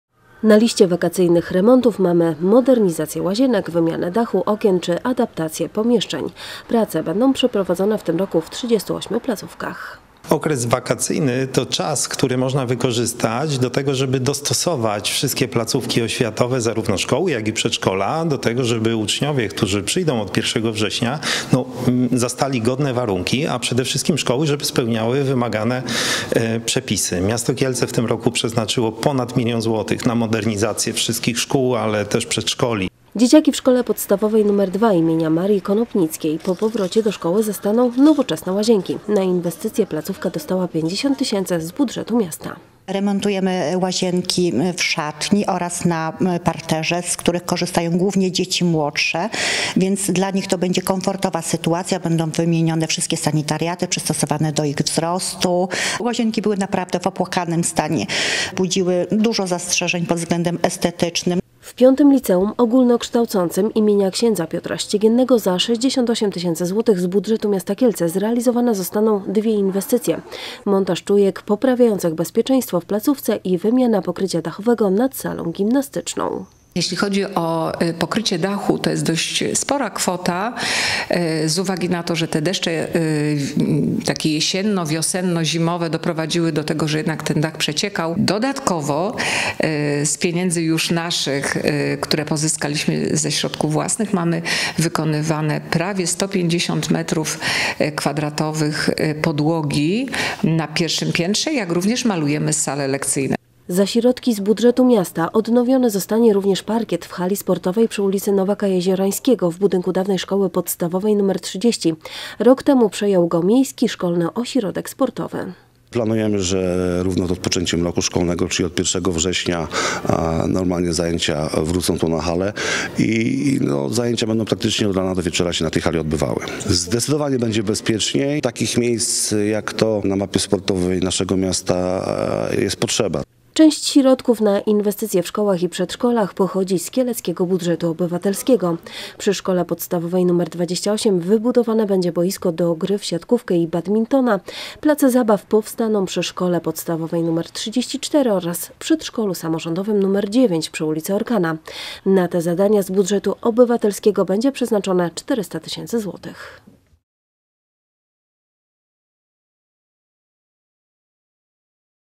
Wakacyjne remonty w szkołach i przedszkolach - ITV Kielce
Miasto wyda w tym roku ponad milion zł na przeprowadzenie prac w kieleckich placówkach oświatowych. Na liście zadań są m.in. modernizacje łazienek, sal gimnastycznych i lekcyjnych, wymiana okien i dachu, czy adaptacja pomieszczeń. Remonty zostaną przeprowadzone w 38 placówkach.